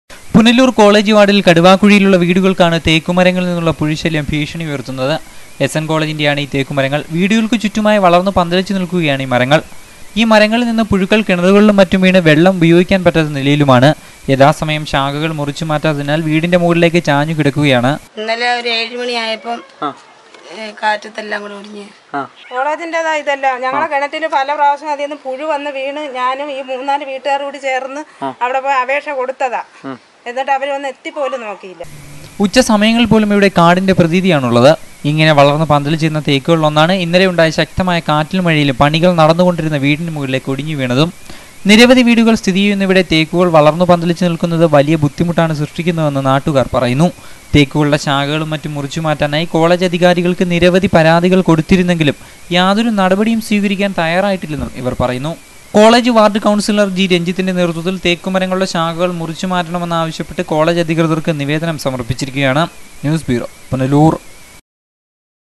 Maggot From Teak Tree(Punalur)
Maggot From Teak Tree in Punalur . college ward people suffering a look because of this